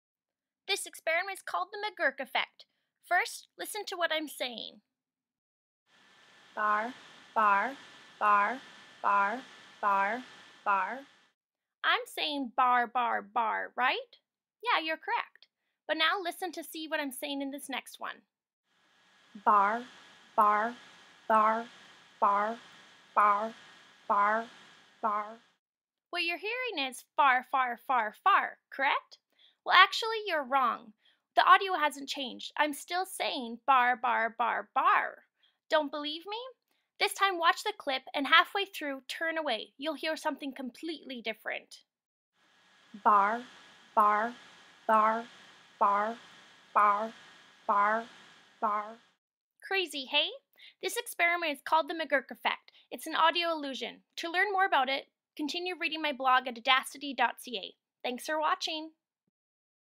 Edacity: The McGurk Effect
Jasmyn takes a look at a crazy audio illusion called The McGurk Effect. But what is it? And what does it have to do with BAR BAR/FAR FAR? Check out Jasmyn's blog to find out more!